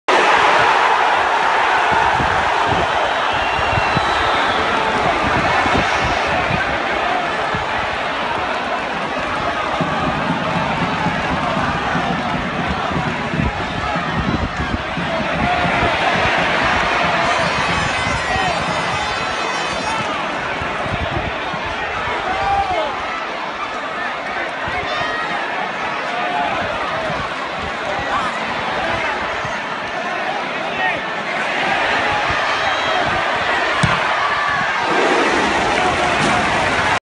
Asedio del Cádiz CF al Bilbao Athletic
Imágenes del Cádiz CF-Bilbao Athletic donde los locales asedian el área rival